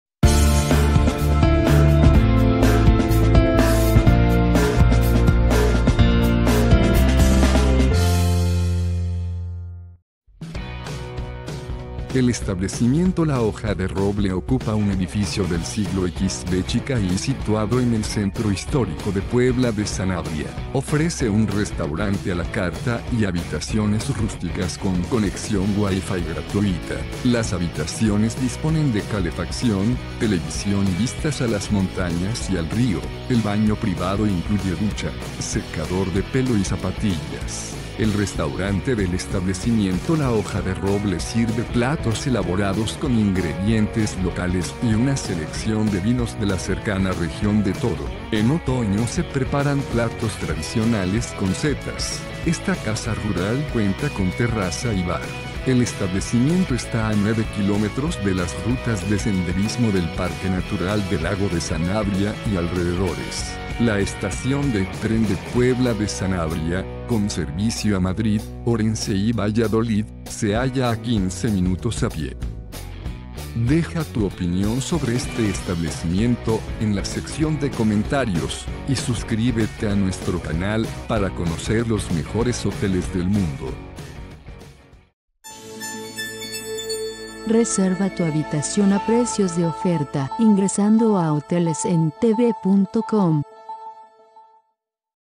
La Hoja de Roble, Casa rural en Puebla de Sanabria
La Hoja de Roble es una Casa rural con bar, recepción 24 horas y prensa  ubicada en la dirección Costanilla, 13 de la ciudad de Puebla de Sanabria, España.